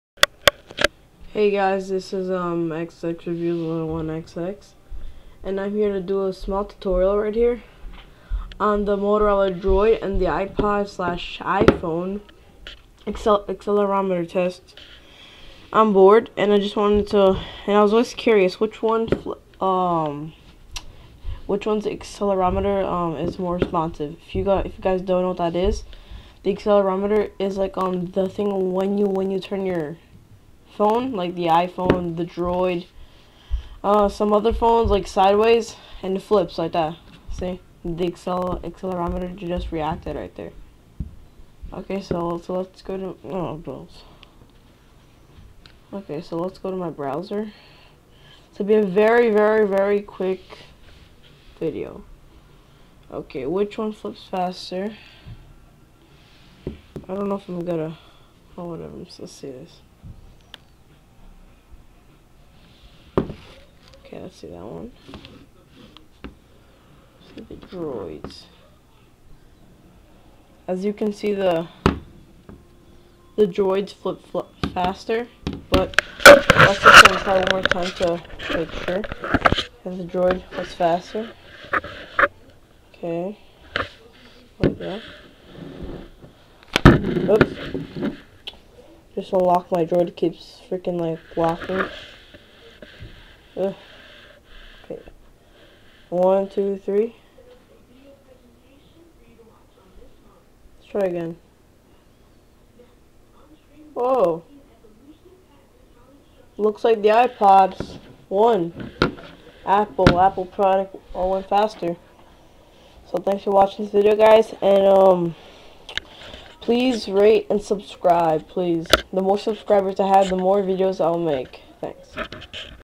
Motorola Droid's and Ipod Touch/Iphone Accelerometer Test
This is a comparison of the Ipod Touch's and the Motorola Droid's Accelerometer.